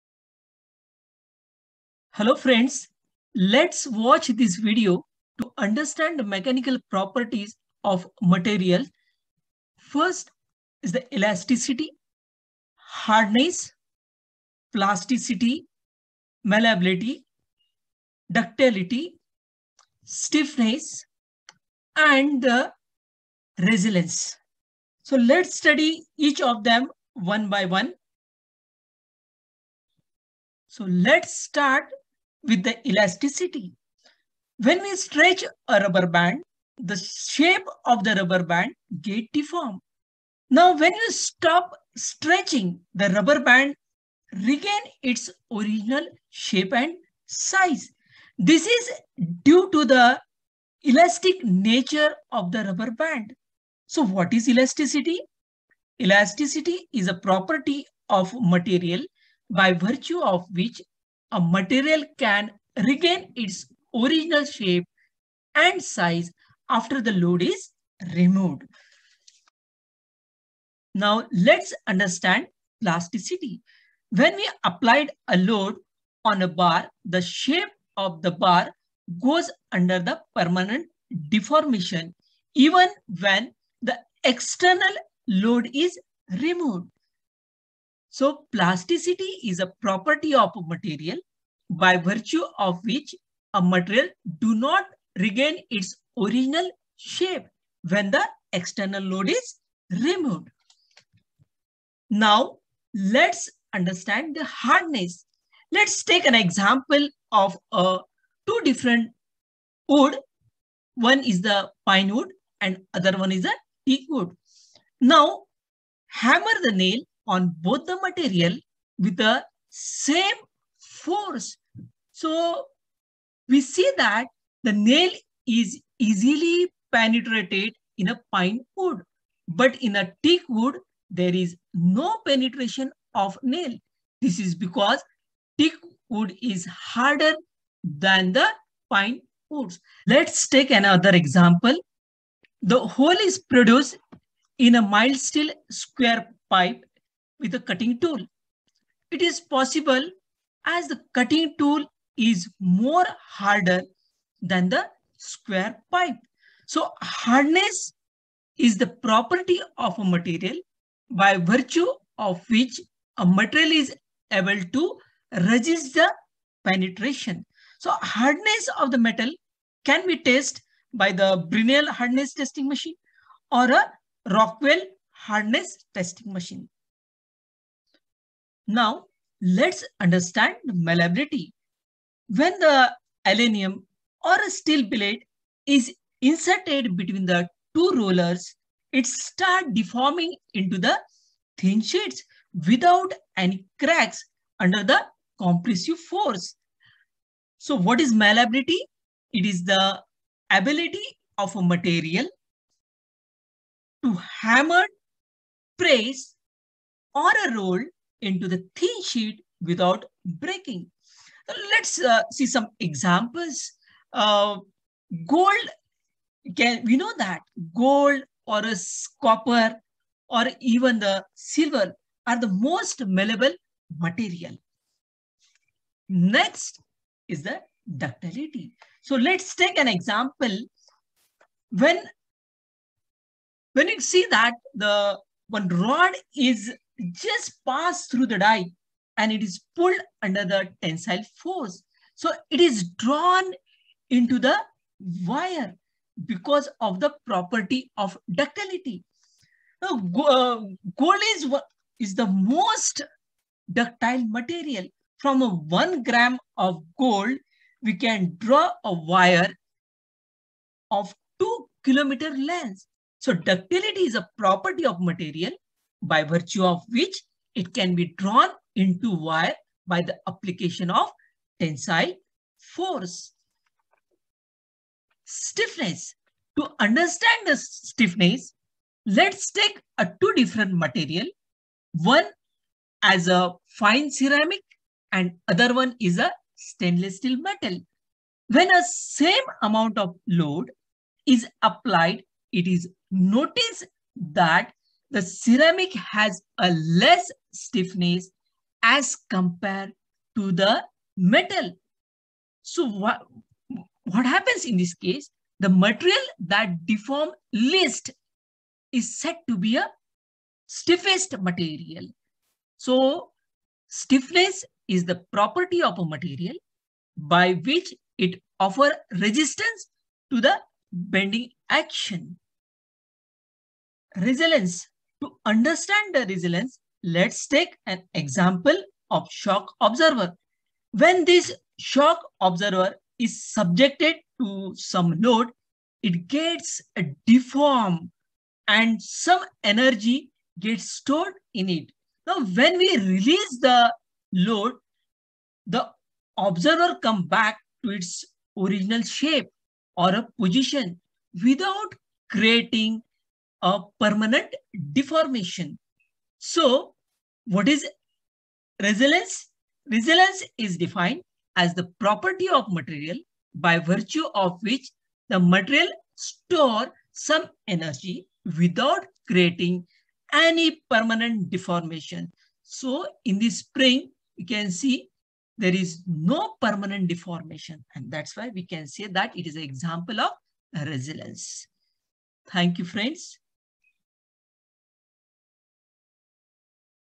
Machine Design- Mechanical Properties of Material: Elasticity, Plasticity, Stiffness| Malleability
Properties of engineering material